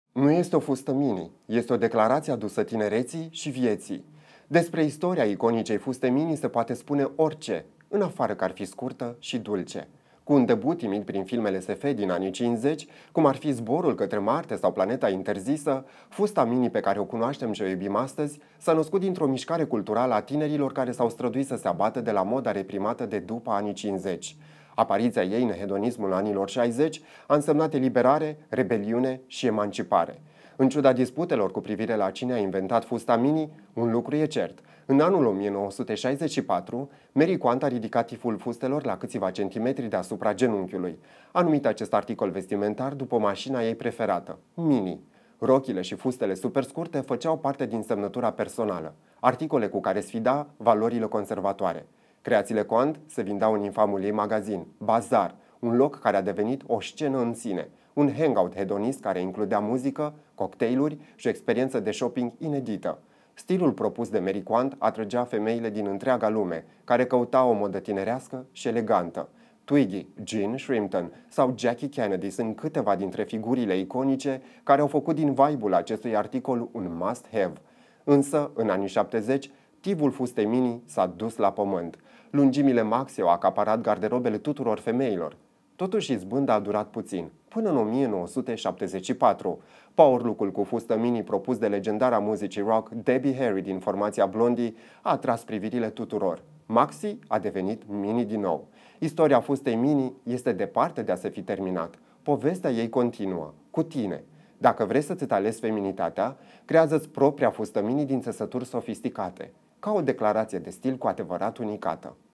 ICONIC. Fusta Mini
Cu un debut timid în filmele SF din anii '50, cum ar fi „Planeta interzisă” sau „Zborul către Marte”, fusta mini, pe care o cunoaștem și o iubim astăzi s-a născut dintr-o mișcare culturală a tinerilor care s-au străduit să se abată de la moda reprimată de după anii '50. Apariția ei în hedonismul anilor '60 a însemnat eliberare, rebeliune și emancipare.